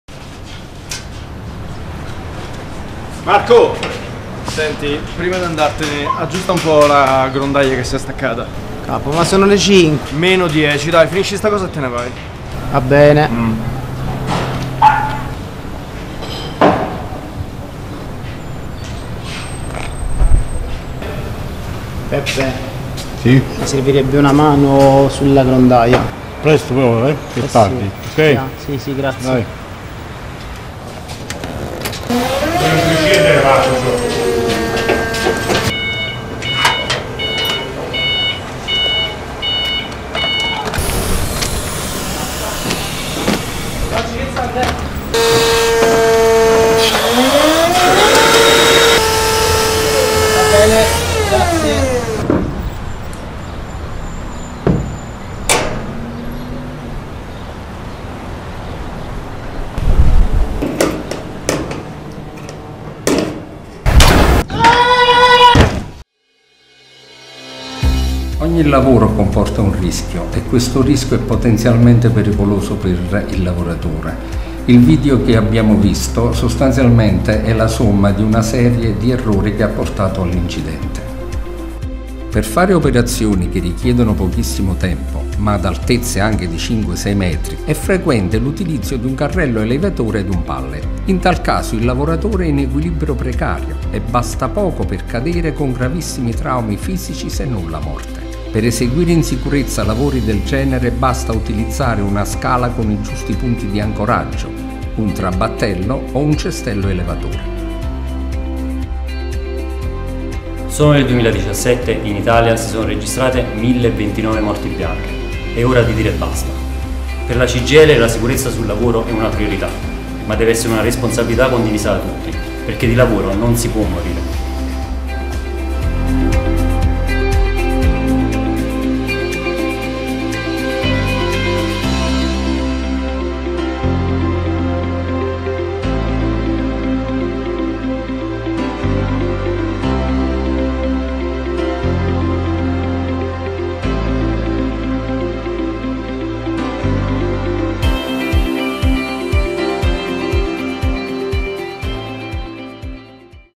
The high fall - South Horizon - Action Movie Design for Nidil CGIL Salerno
Video on safety at work.
High fall with the use of the forklift